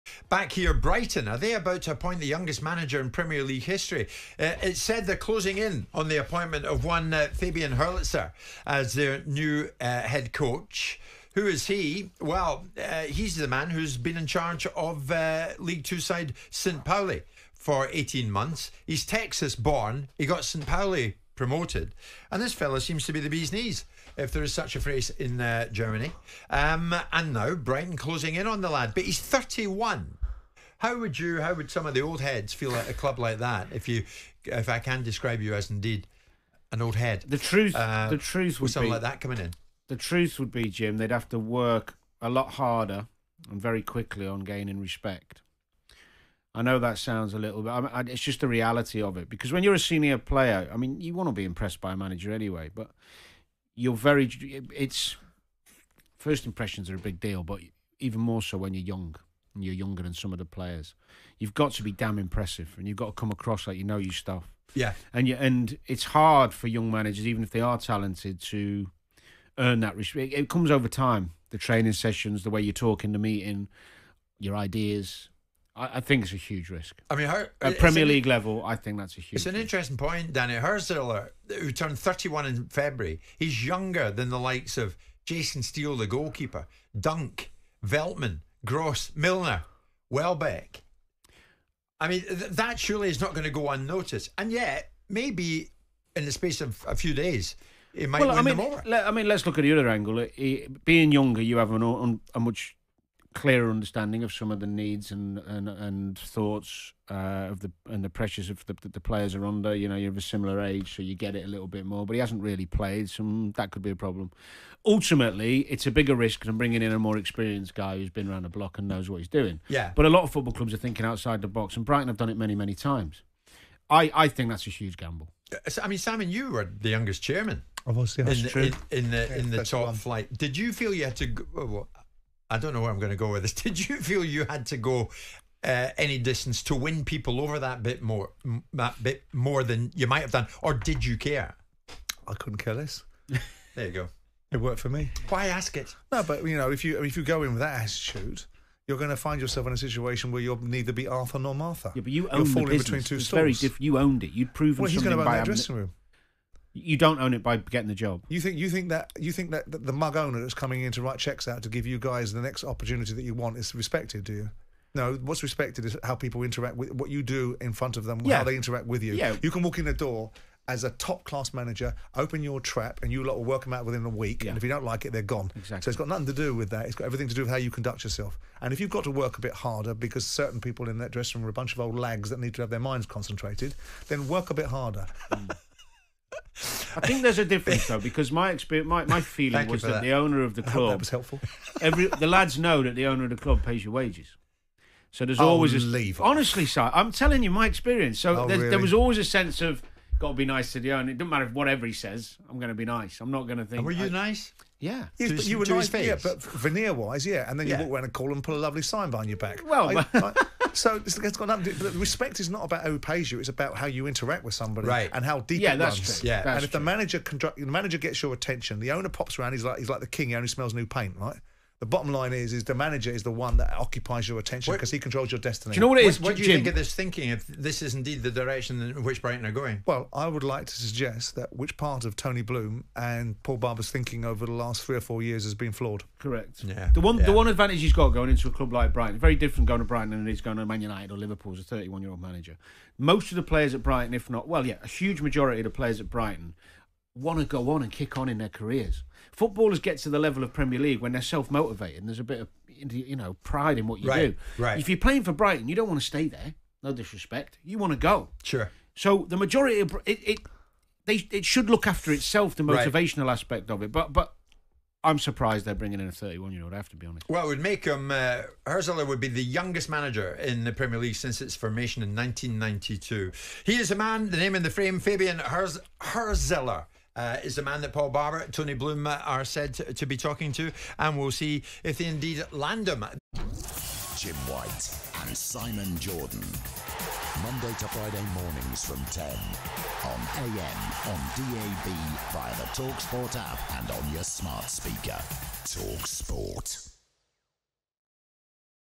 Danny Murphy EXPLAINS Why Fabian Hurzeler Being 31 Is A "HUGE RISK" For Brighton! 👀😬
Danny Murphy, Simon Jordan & Jim White discuss Brighton closing in on Fabian Hurzeler being their new manager and how it could be a risk to the club seeing as he's 31.